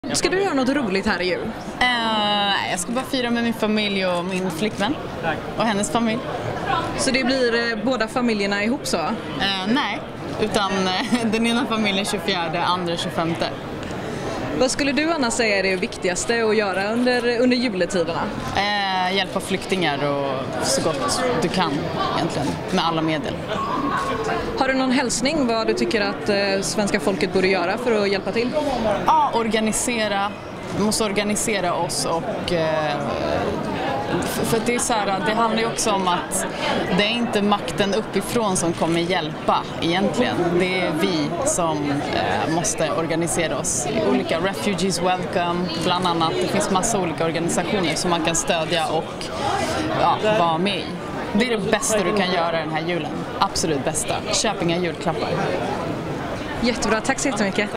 Silvana Imam: "Viktigast i jul är att hjälpa flyktingar – köp inga julklappar""
Artistens uppmaning till svenska folket: "Vi måste organisera oss"